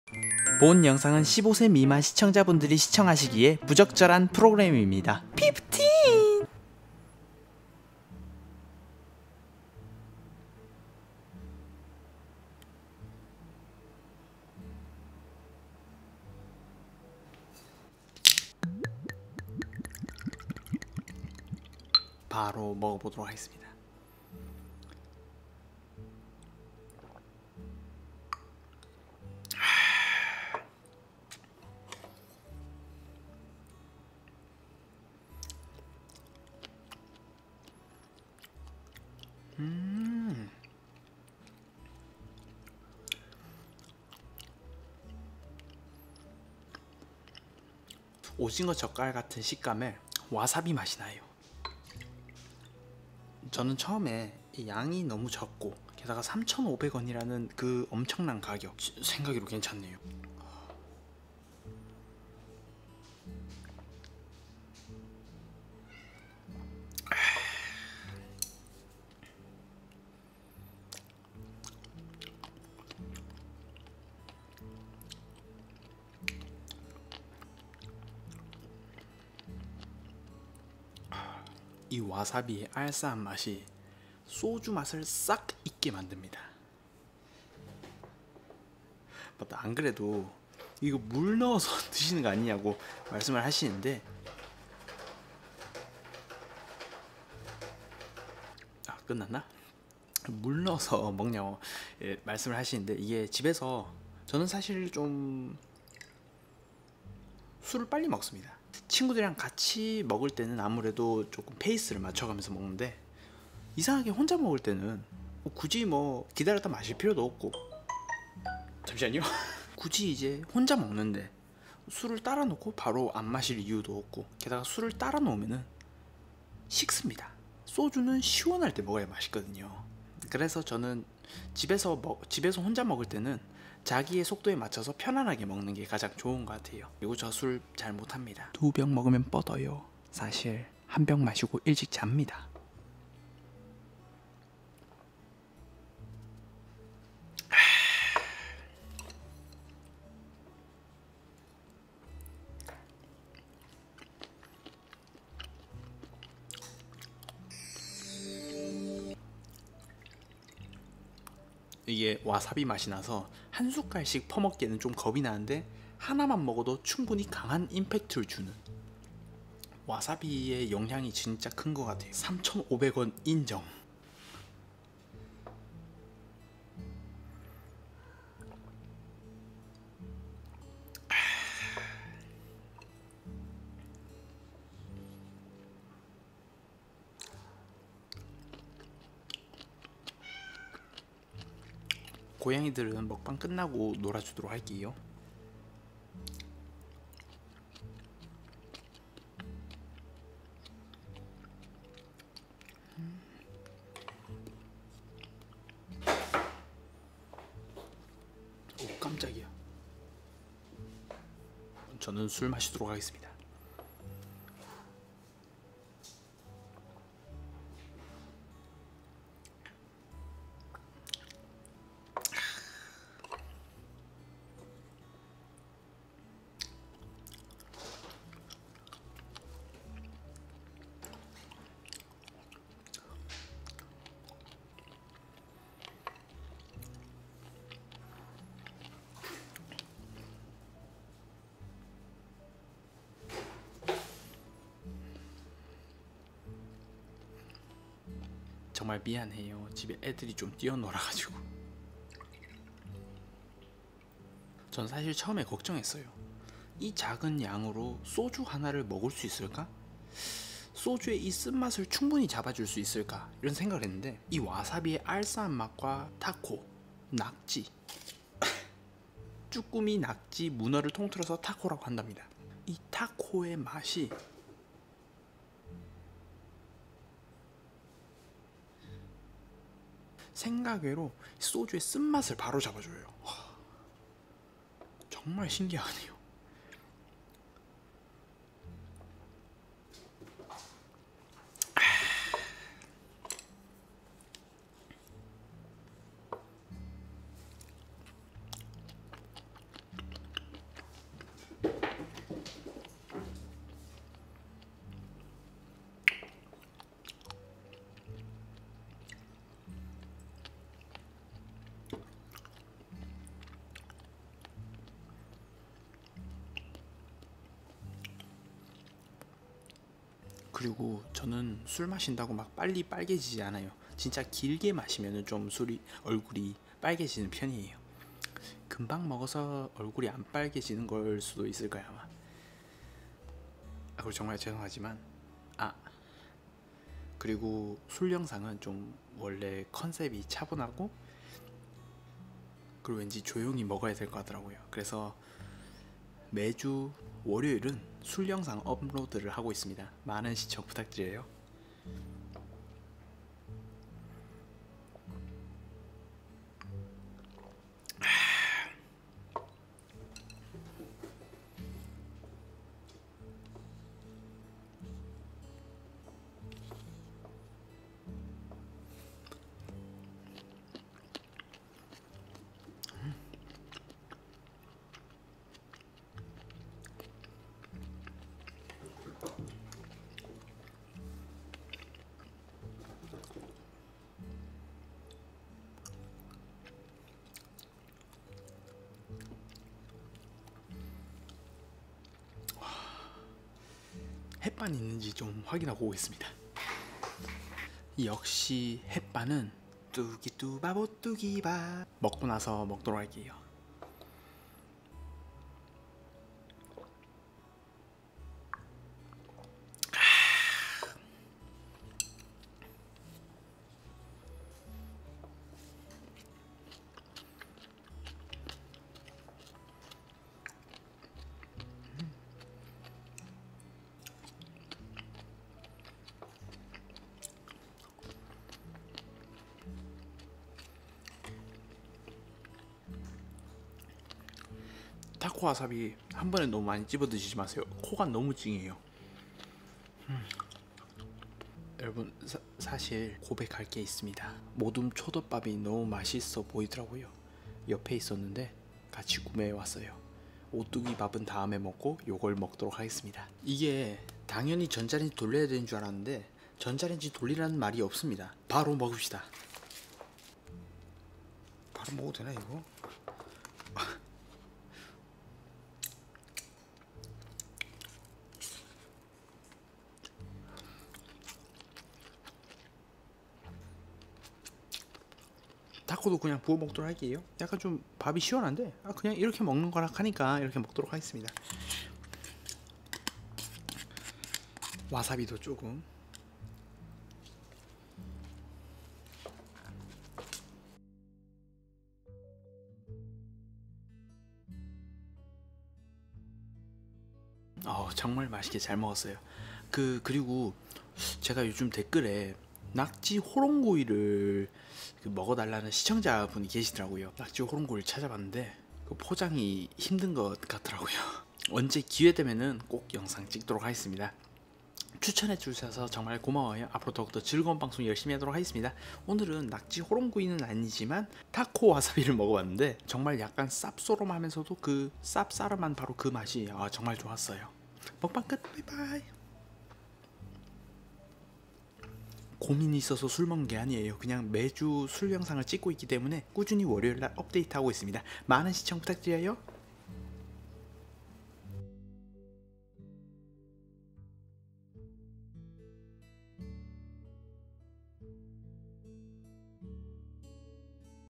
[용사] 오늘 먹을 안주는 와사비타코 입니다!!
gs에서 판매하는 와사비타코입니다!!
가격은 3500입니다 맛있게 잘먹었습니다:)